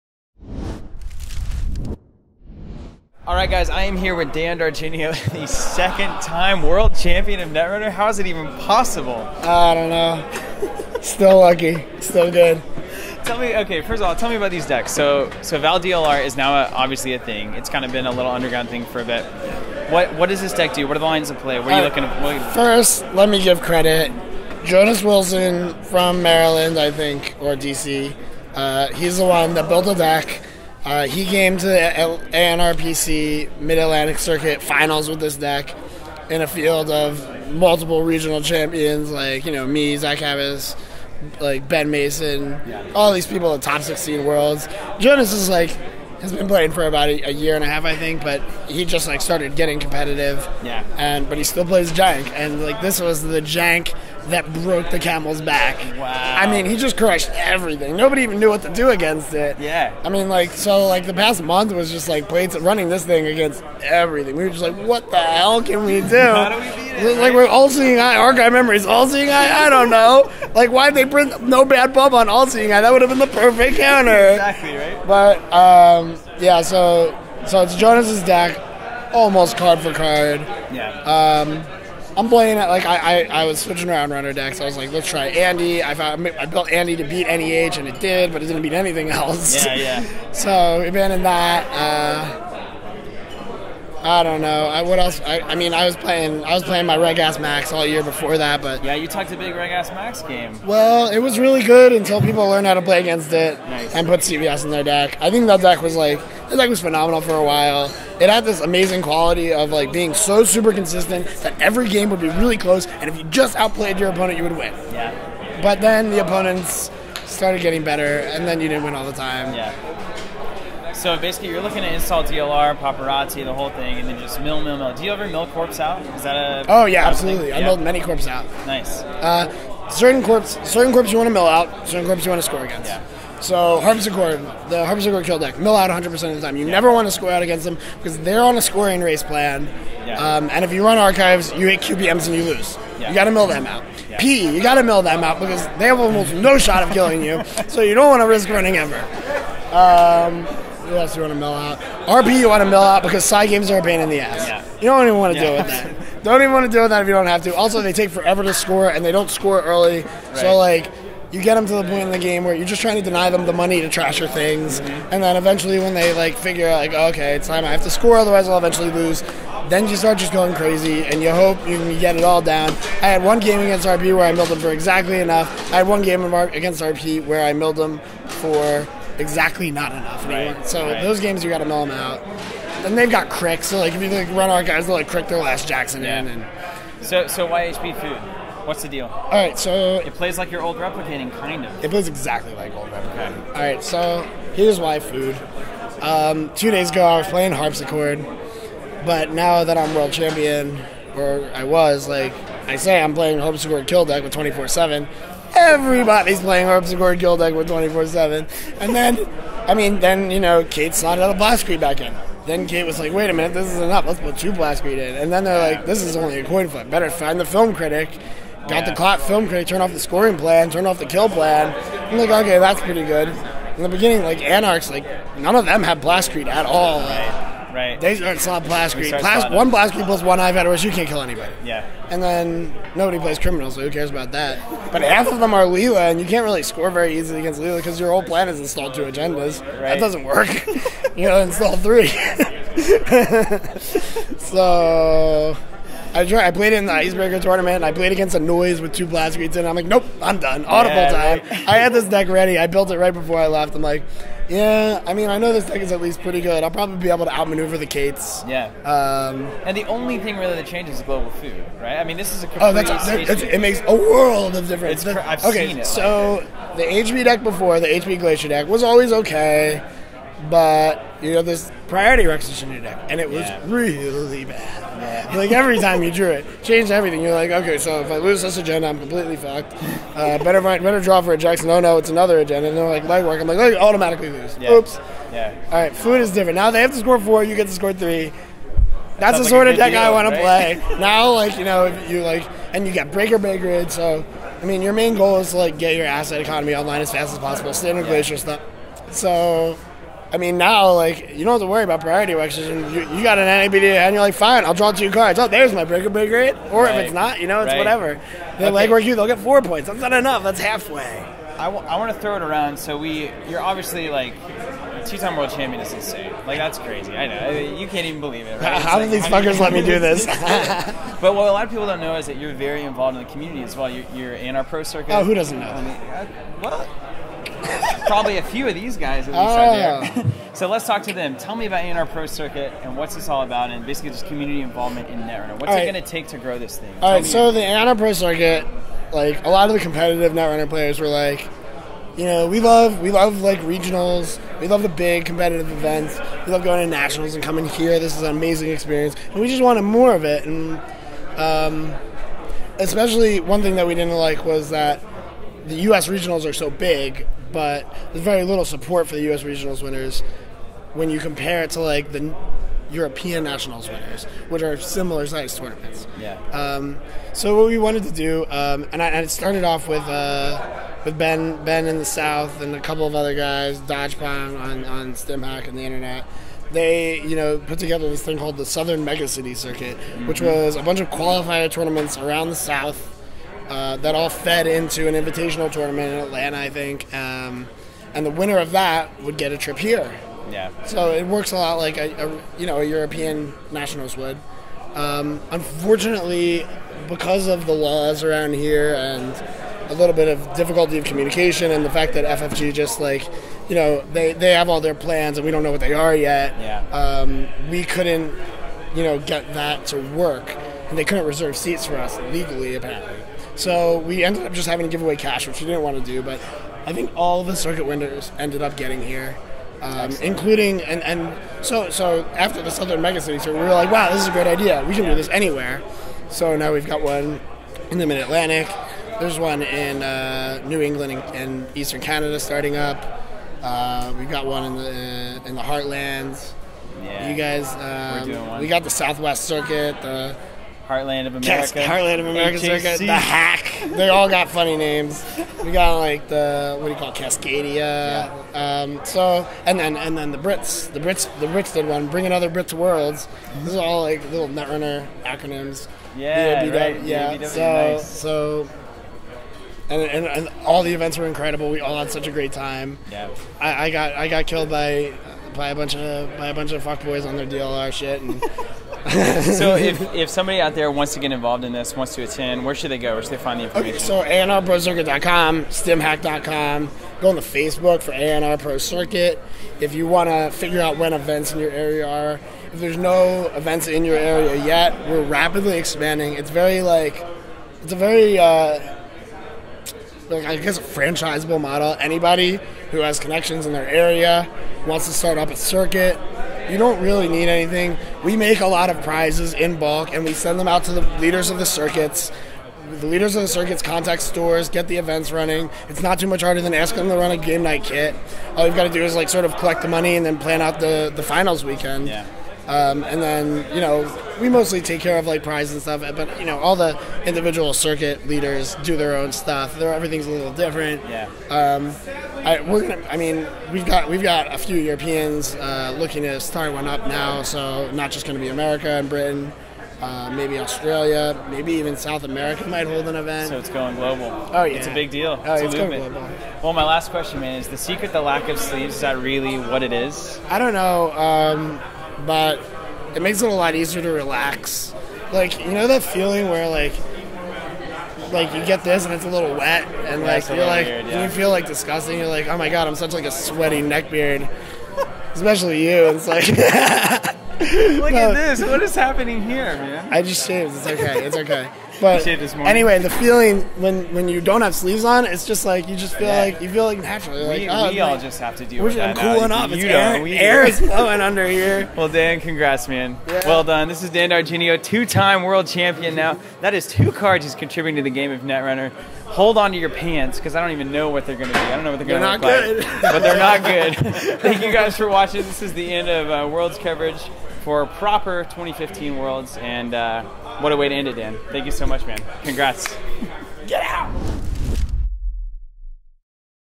Dan D'Argenio Netrunner Interview | FFG Worlds 2015
We have a word with two-time World Netrunner Champion Dan D'Argenio about Worlds, deck archetypes, and the ANRPC. How did we let him do this two times in a row?!

Your support keeps us going! We are perpetually thankful!